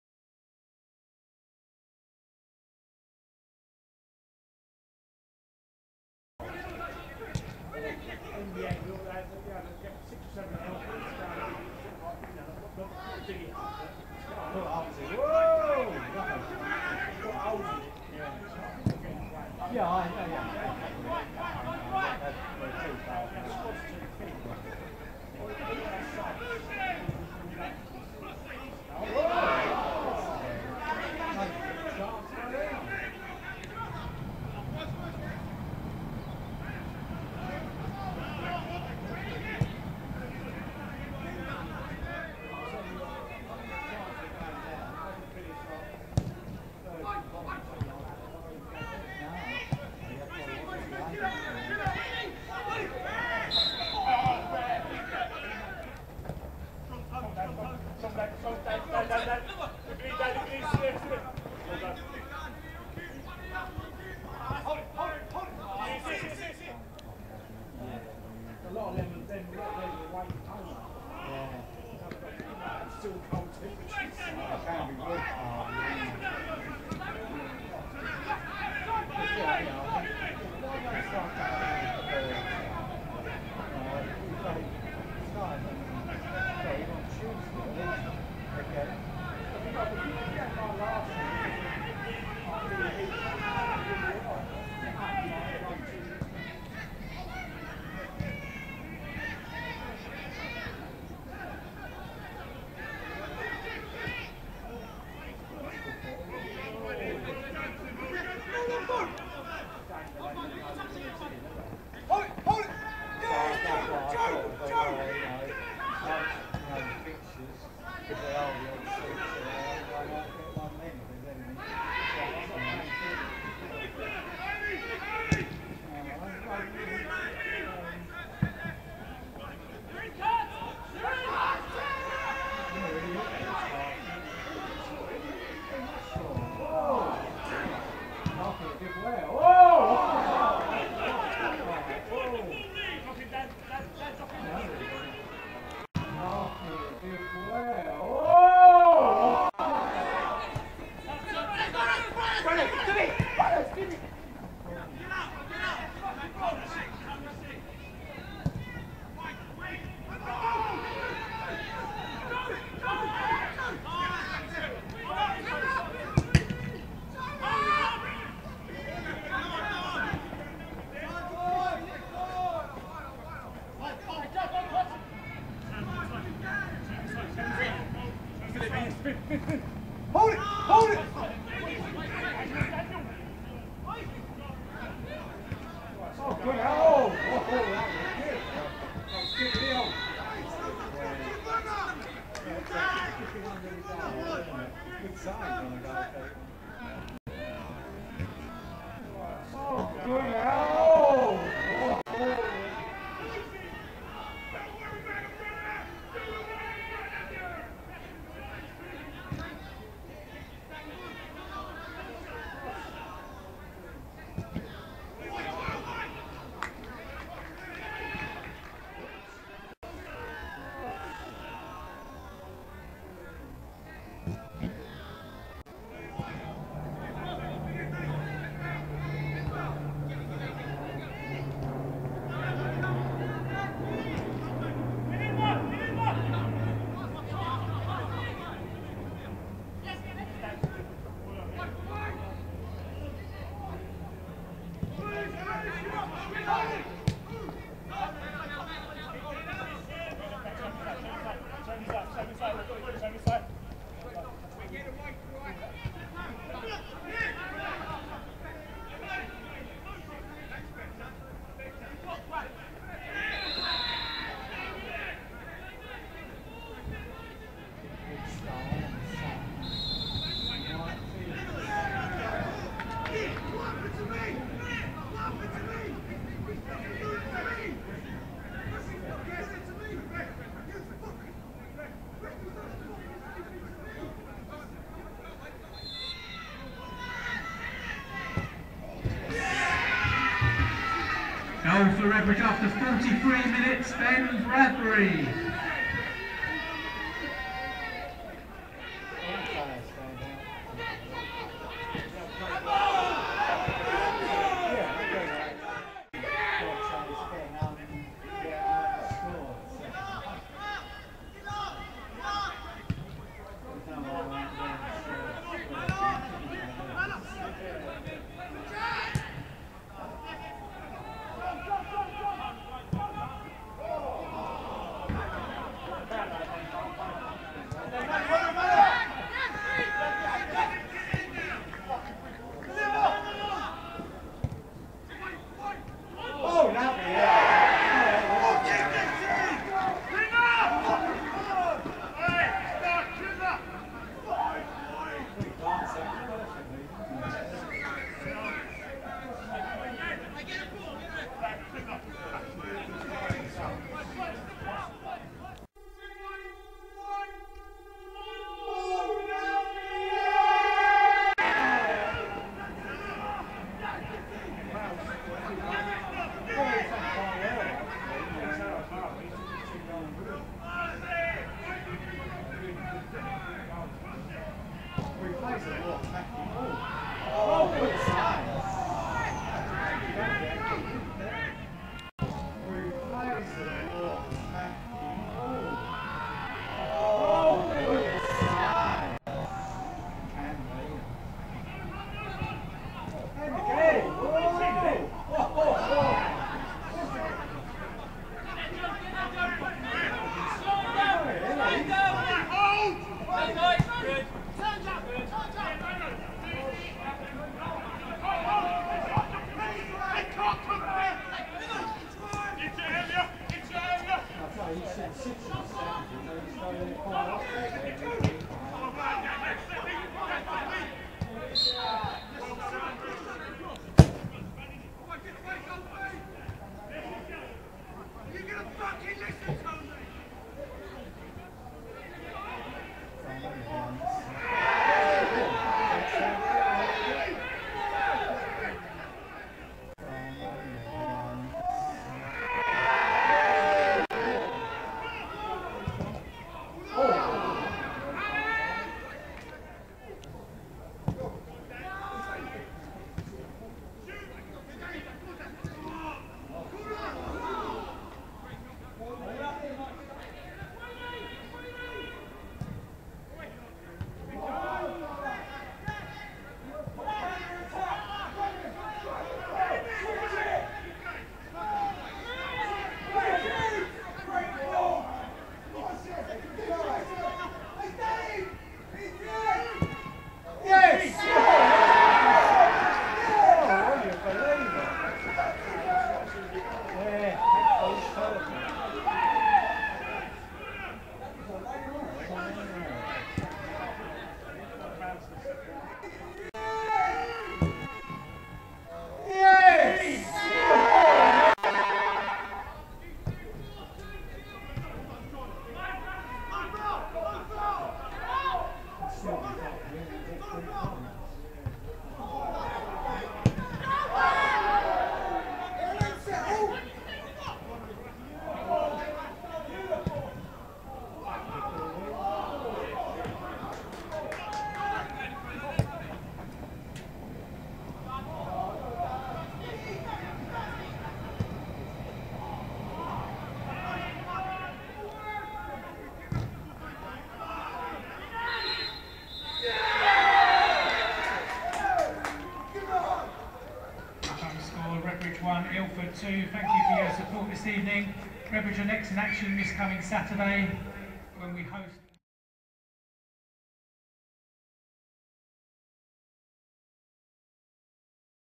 REDBRIDGE V ILFORD
Match highlights from the Ryman Division One (North) local derby game played on Tuesday 17th January 2012.